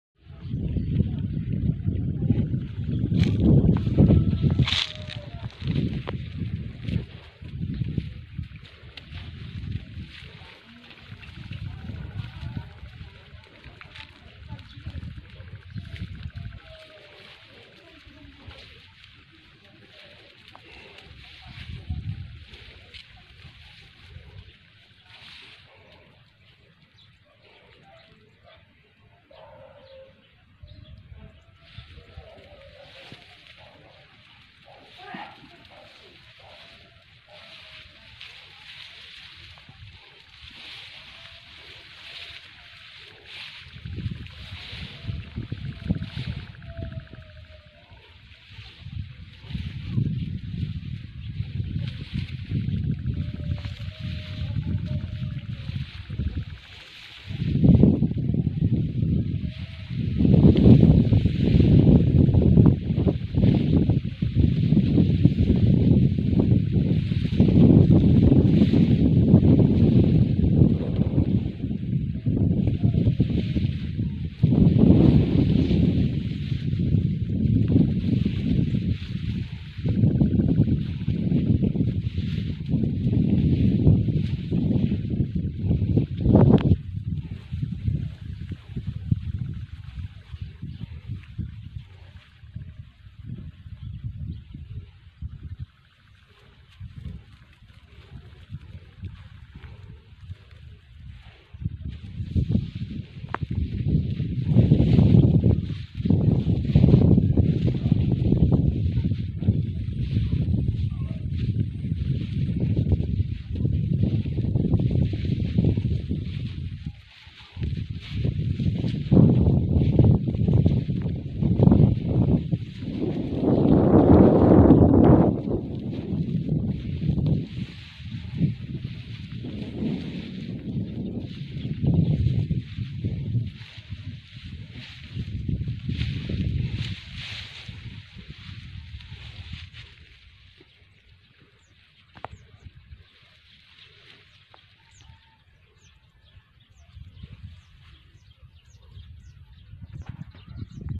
Gaano Nga Ba Katagal Antayin Bago Dumating Sa Hapagkainan Natin Ang Isang Butil Na Bigas o Kanin?
Bawat Butil Ng Bigas O Kanin Ay Mahalaga